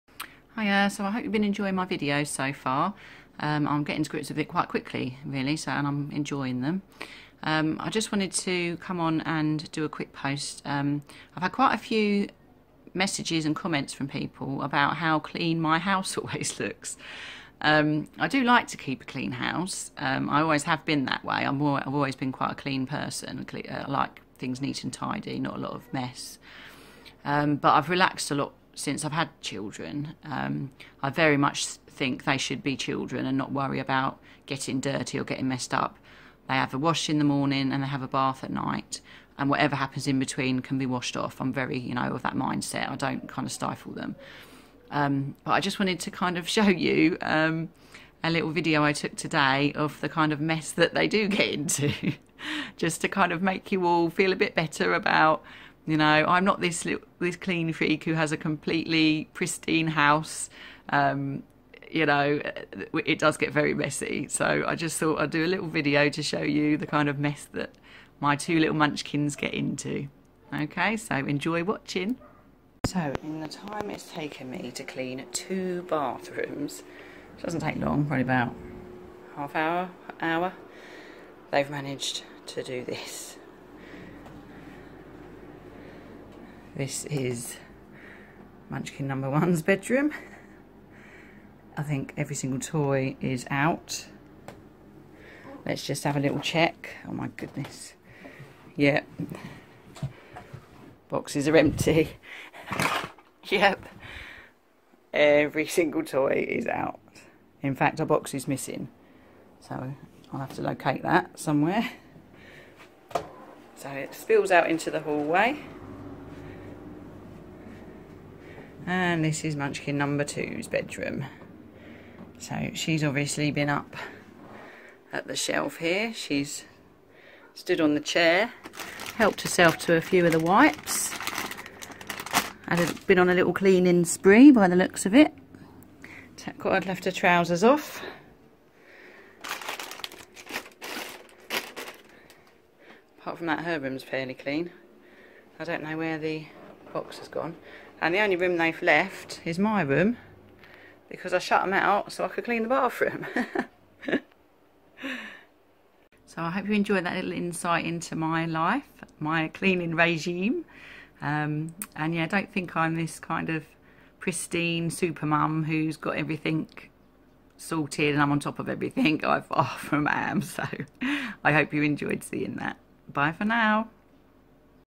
VLOG My house gets messy too!
I've had lots of comments recently about my super clean house, so thought i'd post a little video showing you the kind of mess my munchkins get into! Enjoy! x